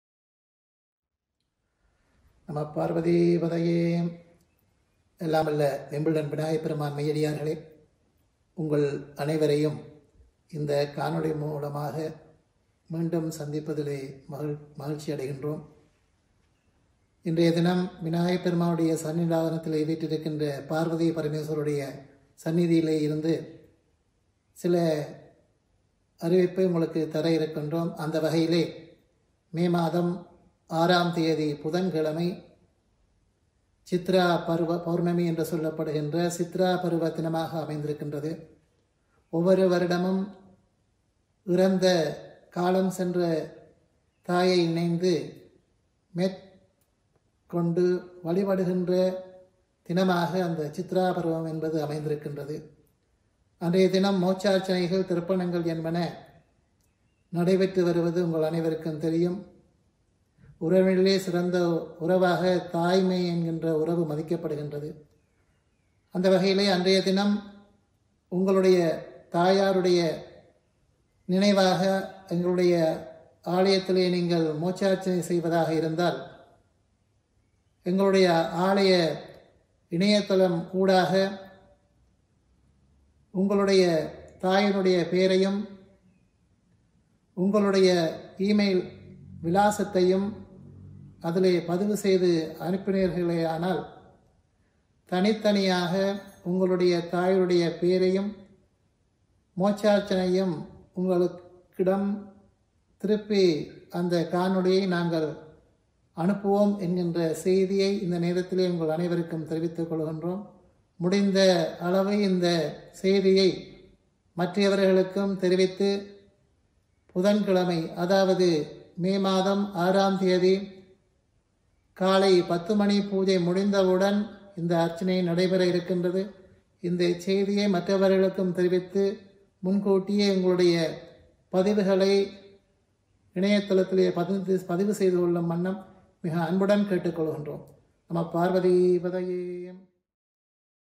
Chitra Paruvum - Message from Vigneswara Kurukkal
On 6 May 2020 we observe Chitra Paruvum. On this day each year we perform a Moksharchanai (personal service of remembrance for departed souls) to devotees’ mothers who have passed away. If you would like to have a ceremony performed on your behalf, please let us know by email ; via the or call the temple.